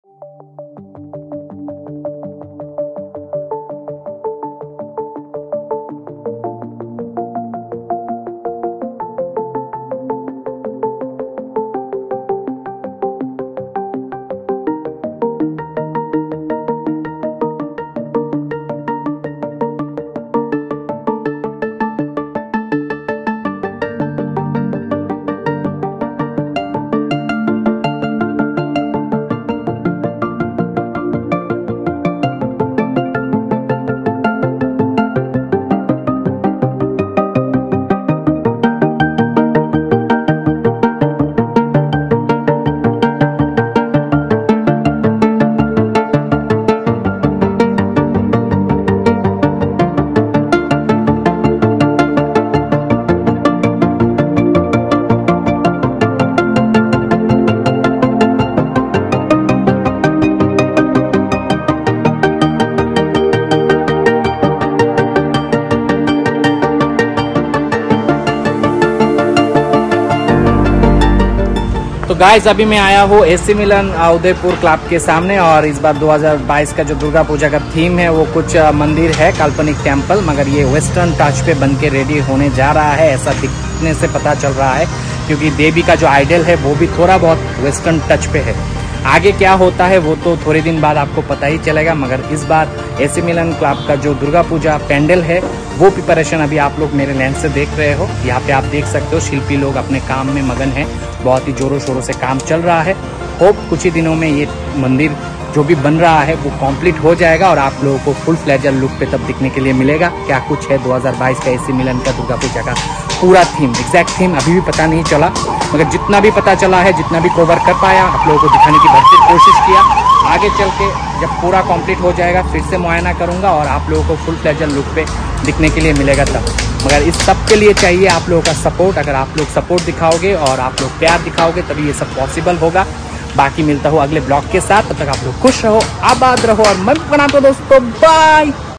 Tripura durga puja 2022 | Udaipur durga puja pandal 2022 | Udaipur durga puja preparation 2022
Tripura durga puja 2022 | Udaipur durga puja pandal 2022 | Udaipur durga puja preparation 2022// Let’s see Tripura durga puja preparation 2022 with me! Udaipur  Club durga puja 2022 theme is intersting. This time this one of the best Udaipur durga puja pandal 2022.